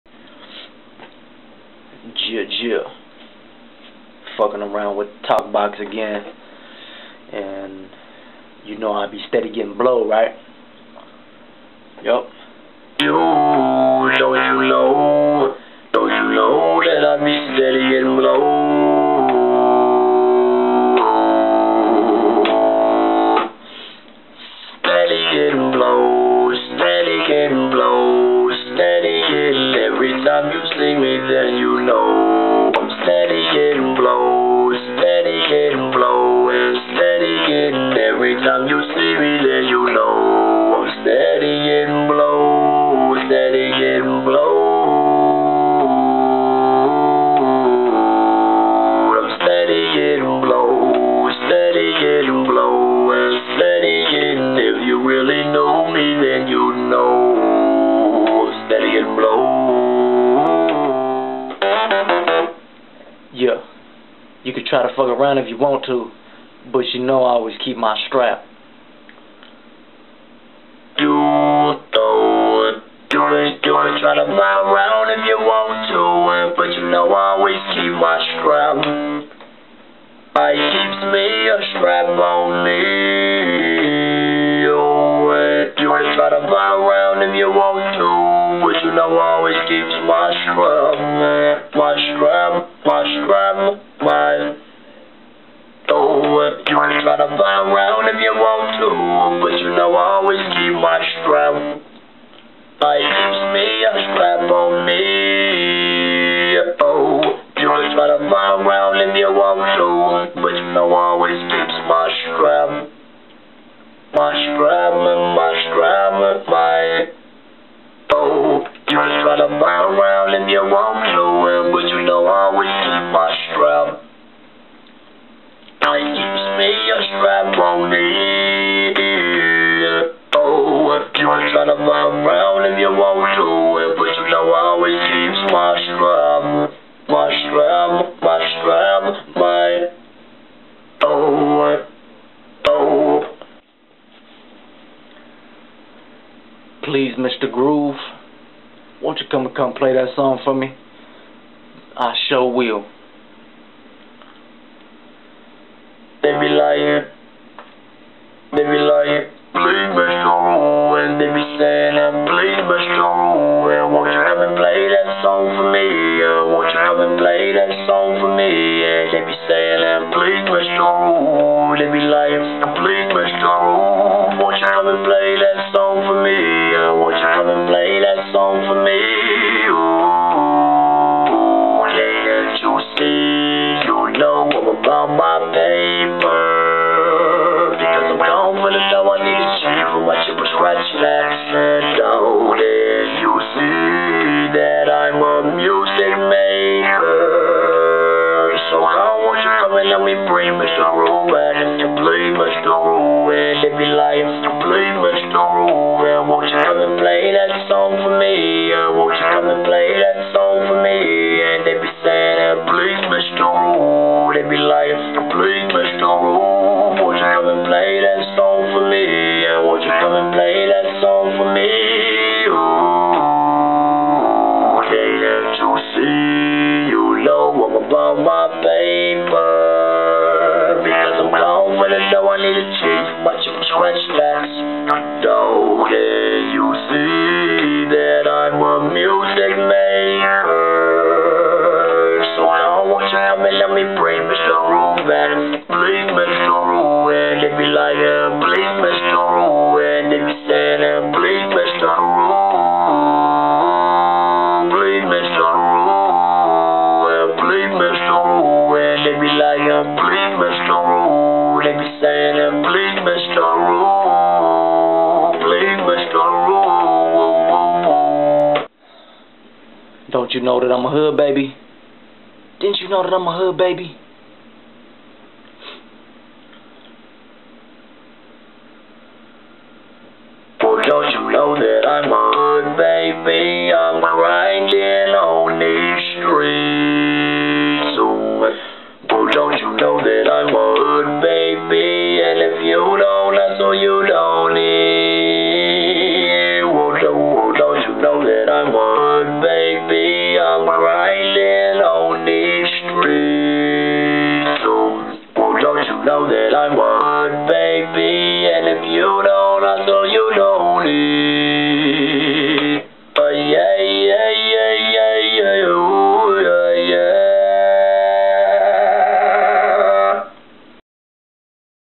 2DASHD USING THE TALKBOX @inde-ground studios
JUST BORED AND SHIT IN MY ROOM ... I WAS SICK  RIGHT HERE ... SO ITS FUNNY I BE WATCHING THE VIDEO I GOT THE SNIFFLES AND SHIT.. BUT THIS IS JUST A MEDLEY OF A FEW DIFFERENT SONGS IVE COME UP WIT USING THE TALKBOX..